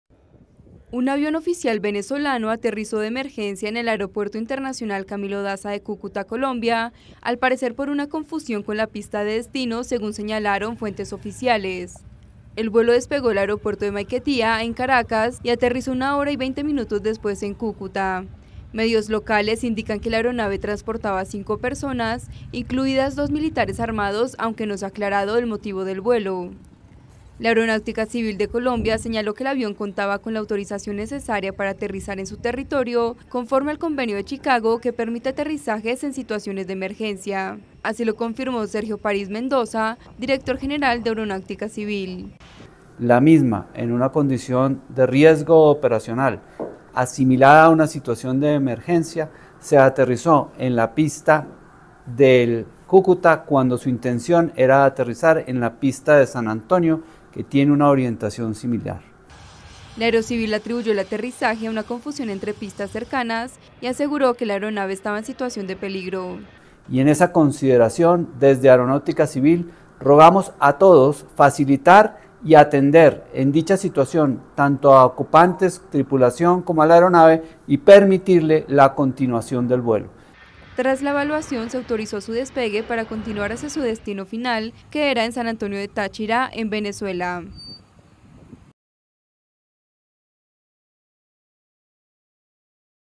El aterrizaje de emergencia de un avión oficial venezolano en Colombia
Un avión oficial venezolano hizo este viernes un aterrizaje de emergencia en el aeropuerto de la ciudad fronteriza colombiana de Cúcuta, al parecer por una confusión con la pista de destino, señalaron fuentes oficiales. El aparato es un Cessna 550 Citation II, con matrícula venezolana YV3226, que aterrizó en el aeropuerto internacional Camilo Daza de Cúcuta, la principal ciudad de la frontera entre los dos países.

"La información que tenemos sobre el aterrizaje del avión venezolano en Cúcuta es que se trata de una emergencia. Esperamos reporte de la Aeronáutica civil que es la autoridad competente para el caso", manifestó en su cuenta de X el presidente colombiano, Gustavo Petro. Según la página de seguimiento de vuelos FlightAware, el avión con la matrícula YV3226 despegó por la mañana del aeropuerto internacional de Maiquetía, en Caracas, y aterrizó una hora y 20 minutos después en el Camilo Daza, de Cúcuta.

Hasta el momento se desconoce el motivo del vuelo y cuántas personas iban a bordo, pero medios colombianos aseguran que la aeronave transportaba a cinco personas, dos de ellos militares armados. La Aeronáutica Civil (Aerocivil) emitió un comunicado en el que indica: "la aeronave YV3226 cuenta con la debida autorización por parte de la autoridad de aviación civil colombiana" conforme a lo establecido en el Convenio de Chicago, que "reconoce el derecho a realizar escalas técnicas el territorio colombiano, así como la utilización de aeródromos en situaciones de emergencia".

Al parecer el destino del vuelo era la vecina ciudad venezolana de San Antonio del Táchira, situada en la frontera, pero la tripulación tuvo que aterrizar en Cúcuta. Según la Aerocivil, en este caso el aterrizaje de emergencia fue "derivado de una circunstancia de confusión entre pistas cercanas" lo que confirma la versión de que el avión se dirigía a San Antonio del Táchira, ciudad separada de Cúcuta solo por el río Táchira.

La Aerocivil agregó que se le dio permiso de aterrizar porque el hecho "corresponde a una aeronave que estuvo en situación de peligro". "En consideración al derecho internacional que en dicha condición tiene la aeronave YV3226, a la información recopilada sobre el incidente operacional, se autoriza el despegue de dicha aeronave para continuar su vuelo a su destino final", agregó la Aerocivil.